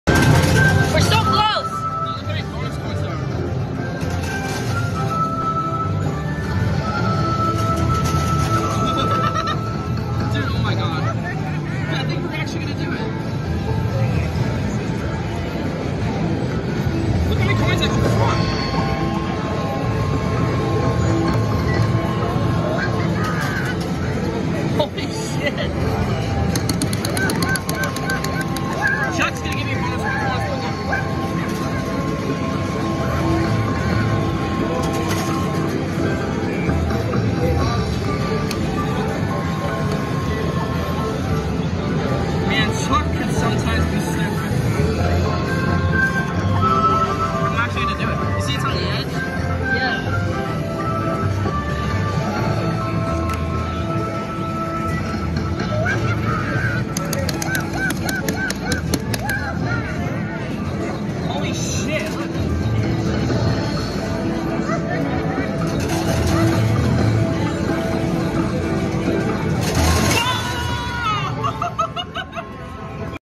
Coin Pusher Tower of Coins
I wasted 370 credits on this game.